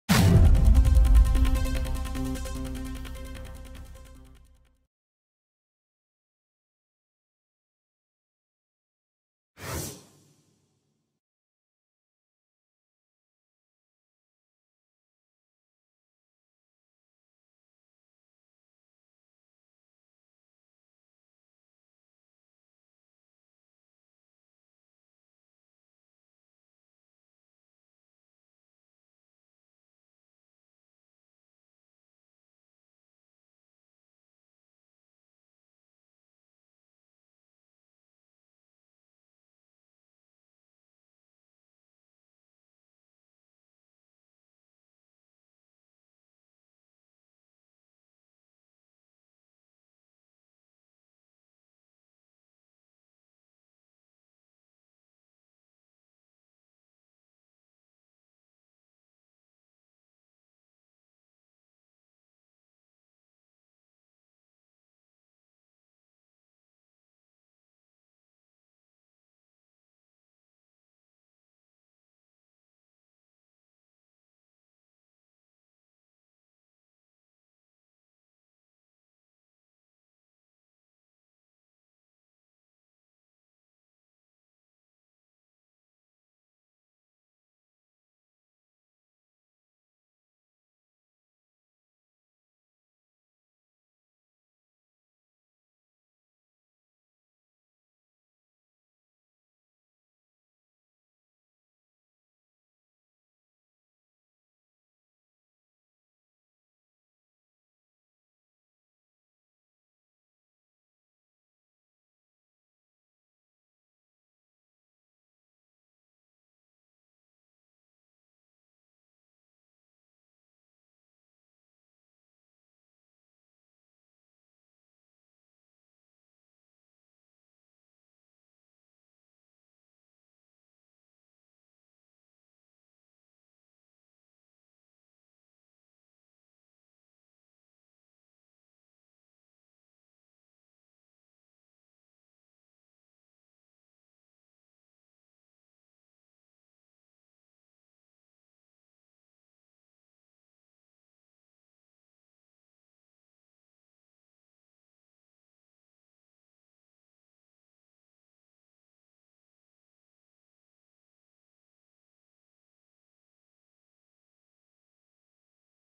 NIPSEY HUSSLE ALLEGED KILLER LAWYER CHRISTOPER DARDEN QUIT ERIC HOLDER CASE
The lawyer for Eric Holder aka shity cuz is no longer on the case.
 CHRISTOPER DARDEN the prosecutor in Oj Simpson case who created a stir after deciding to represent Eric Holder who has been charge with murder of Nipsey Hussle...STOP BY NEWS